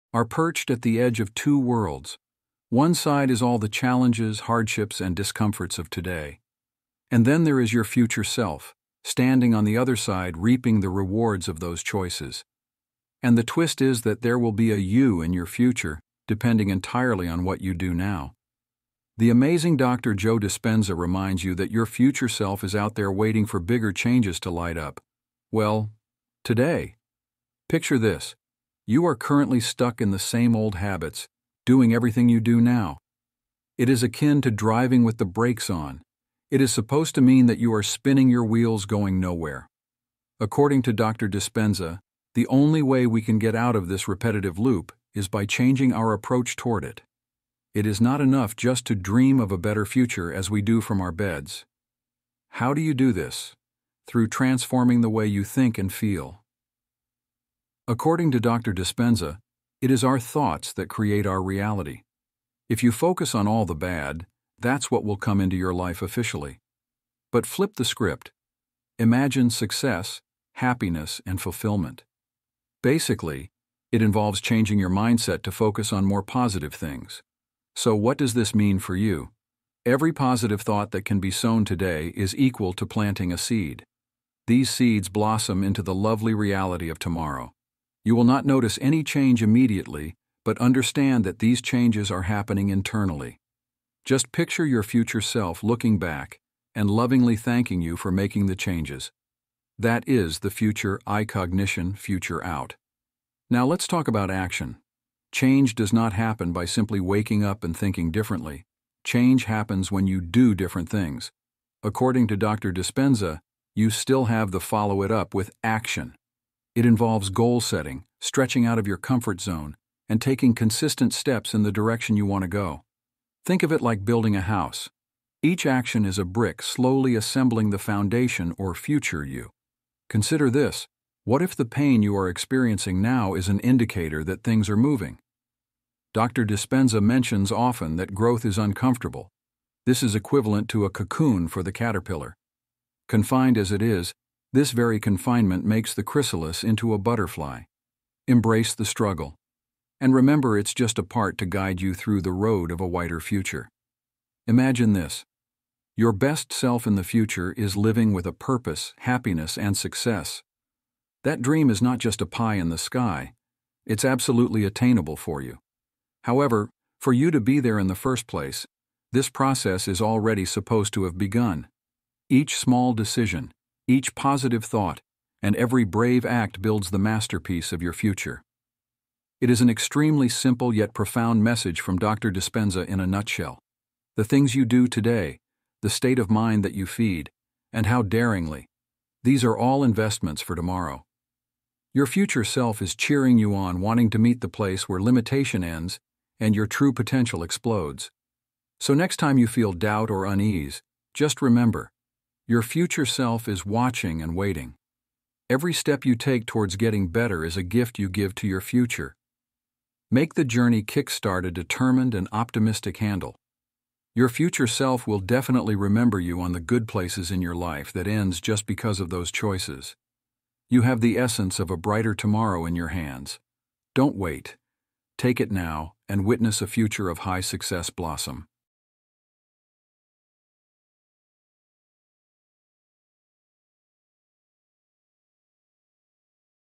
This Will Change Your Life FOREVER! - Dr. Joe Dispenza Motivation
"This Will Change Your Life FOREVER! - Dr. Joe Dispenza Motivation": Imagine unlocking the hidden potential within you, tapping into the limitless power of your mind to reshape your reality. Dr. Joe Dispenza reveals how mastering your thoughts can transform every aspect of your life, leading you to a future beyond your wildest dreams.